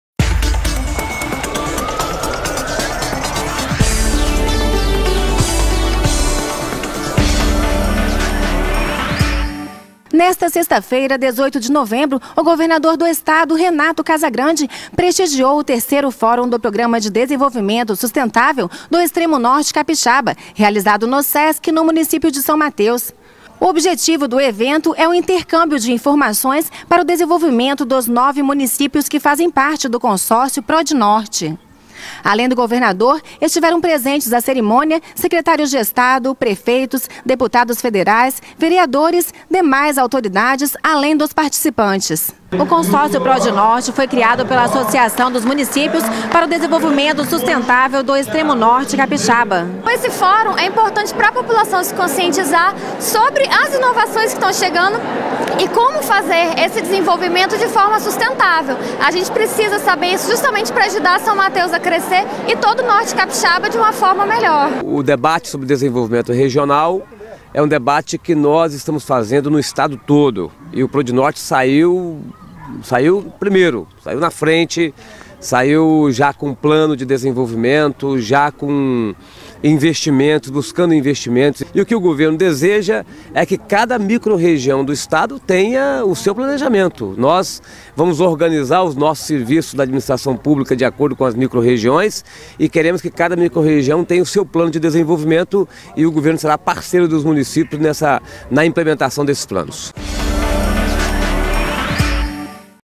Casagrande reafirma que Governo do ES quer microrregiões organizadas
Governador diz que debates orientam atuação para desenvolvimento regional equilibrado no Espírito Santo
Renato Casagrande reafirma que Governo do ES quer microrregiões organizadas e com planos de desenvolvimento específicos